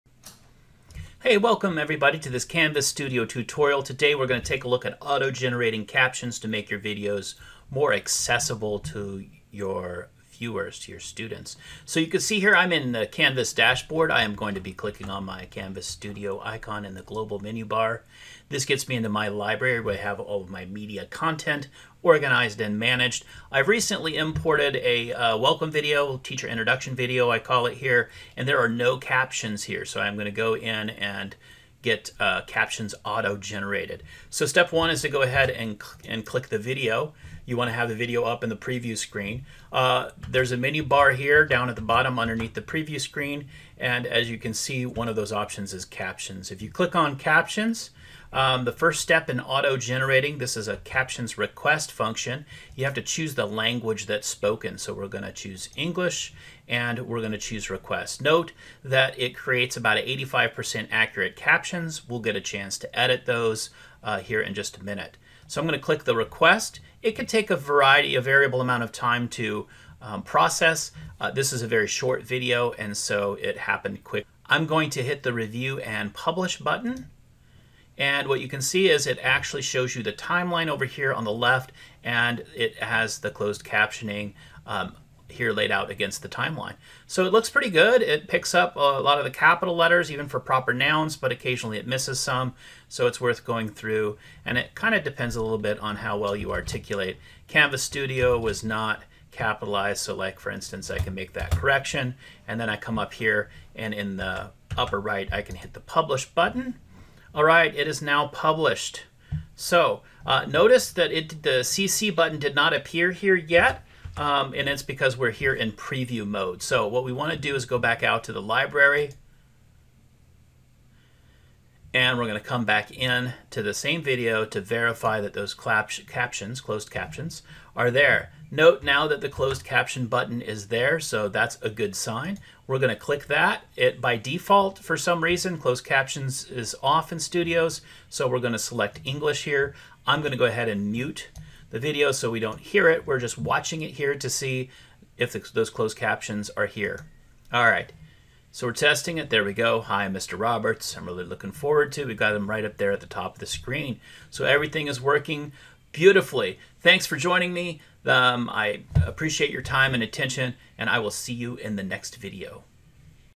Auto Generate Closed Captions of Your Videos in Canvas Studio
In this quick tutorial, we explore how to use Canvas Studio to auto-generate closed captions for videos you have created using Canvas Studio (you cannot auto-generate closed captions for external videos).